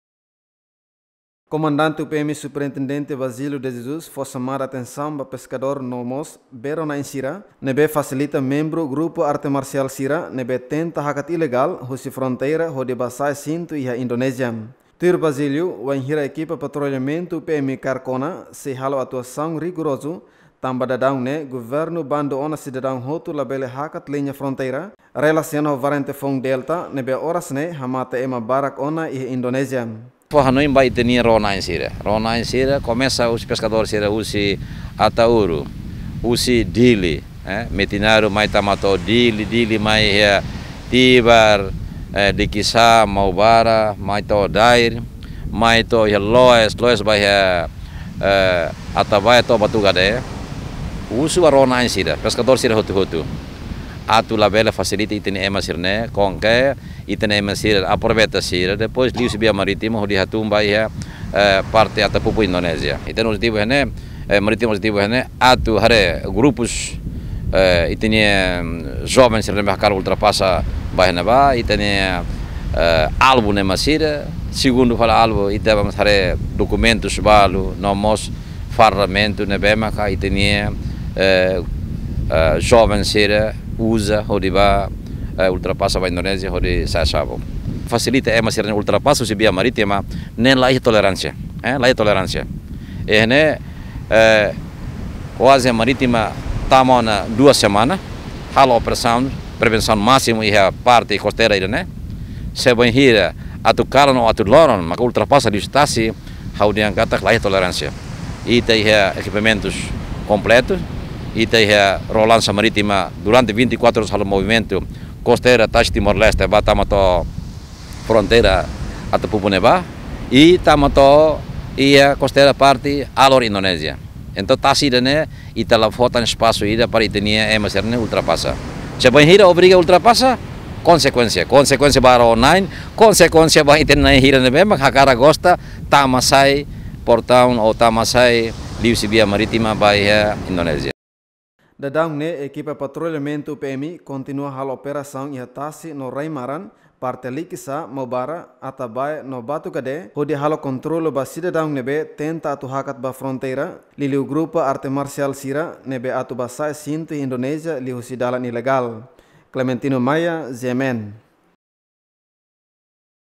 UPM husu Bero nain labele fasilíta GAM tama ilegál ba hasae sintu iha Indonezia
UPM husu Bero nain sira iha Fronteira labele fasilíta Grupo Arte Marsiál tama ilegál ba hasae sintu iha Indonezia